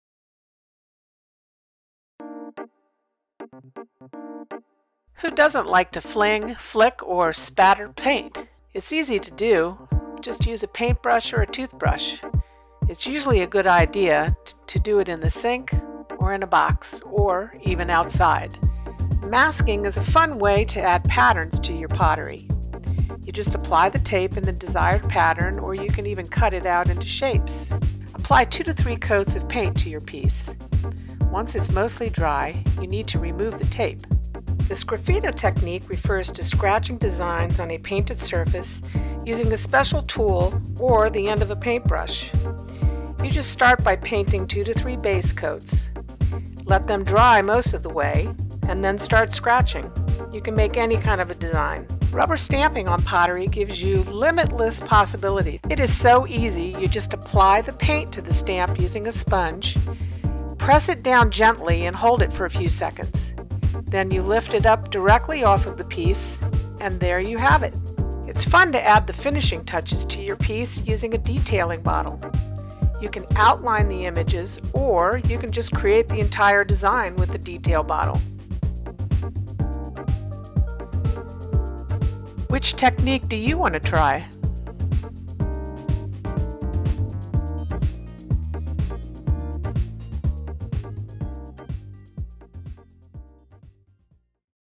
Clay Cafe Falls Church Pottery Painting Techniques Technique Tuesday
Clay Cafe Studios Falls Church video showing a few simple pottery painting techniques for Technique Tuesday.